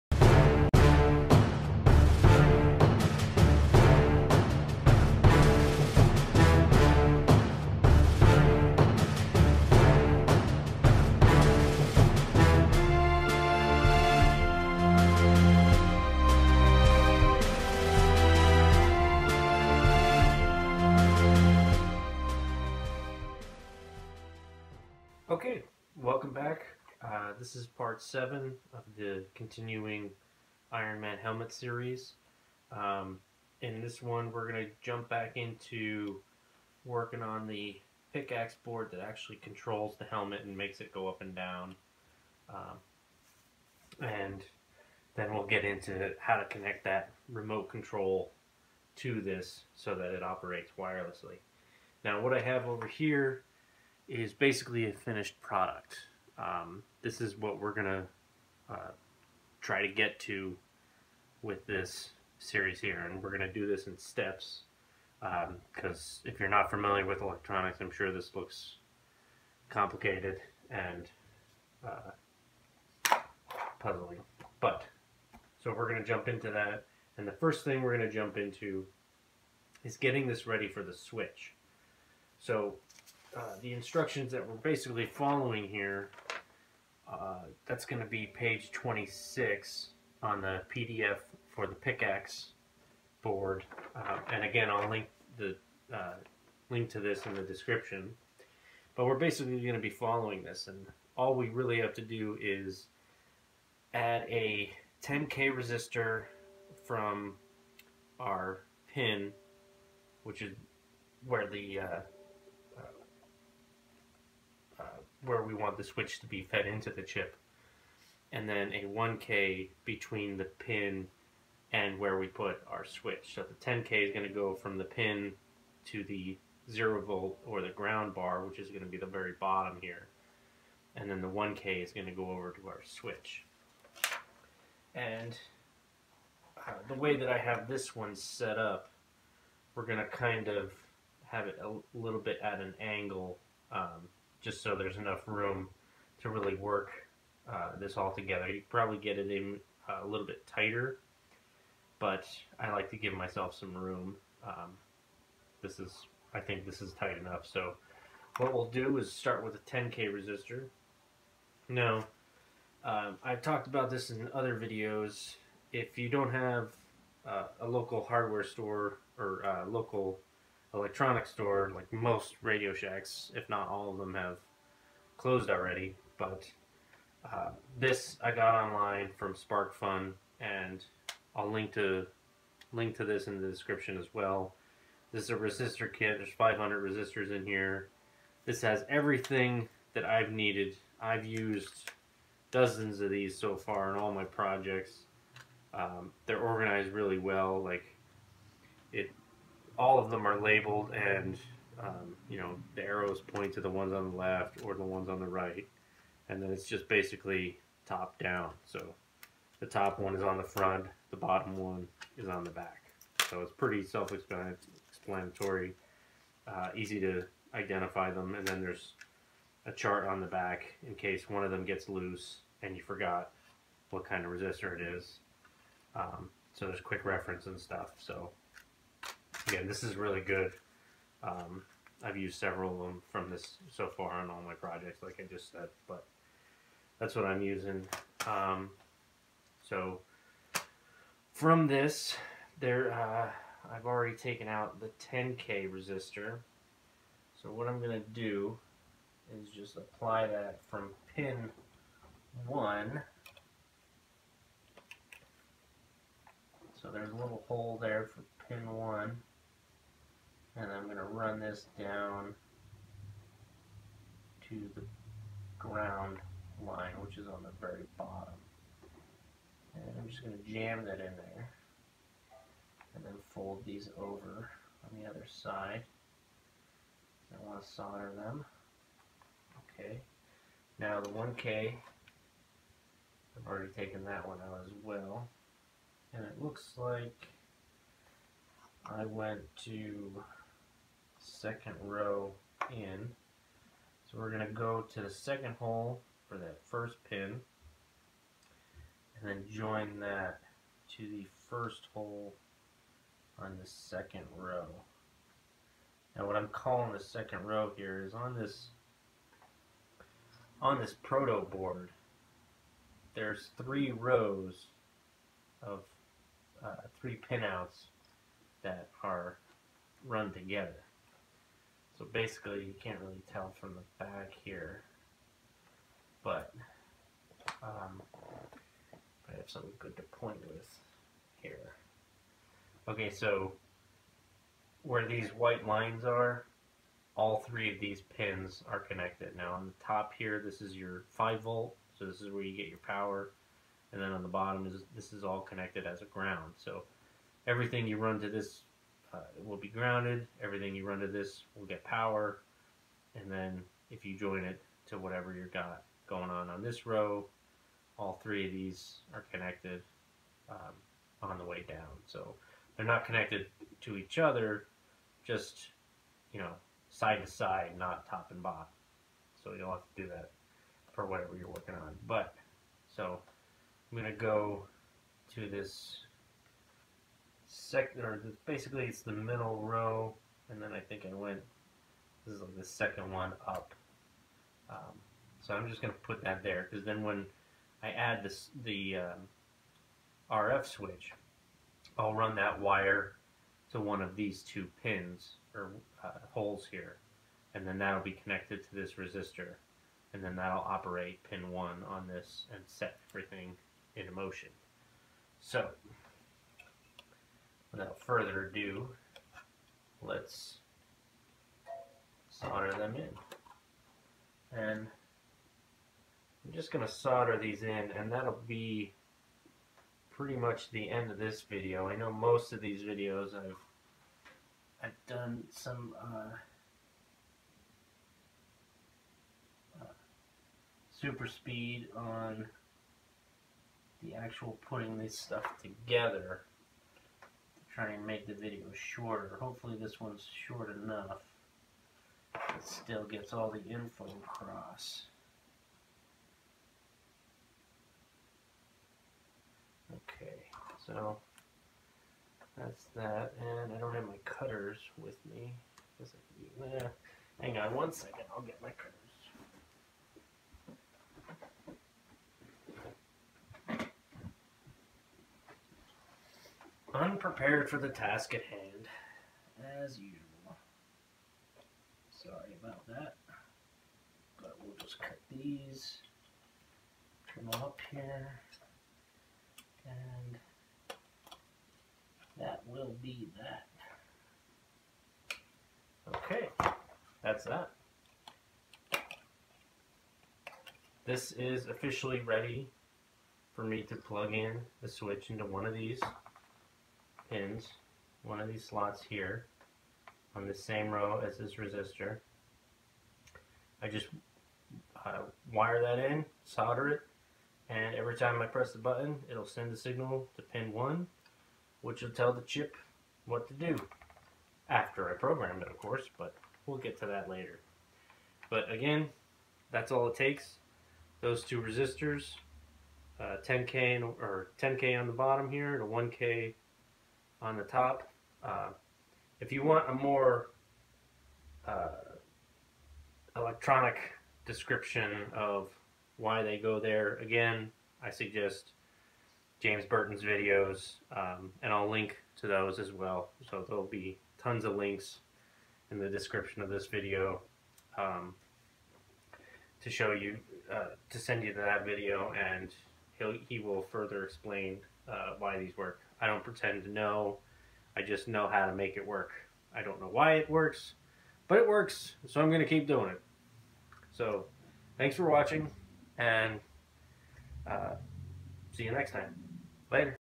Iron man helmet wireless controller: Part 7 switch circuit
This video covers video covers installing the switch circuit to the picaxe proto board that will actually control the servos of the faceplate.
I strongly advise that you check each video before placing an order for anything covered here to save yourself too many shipping fees.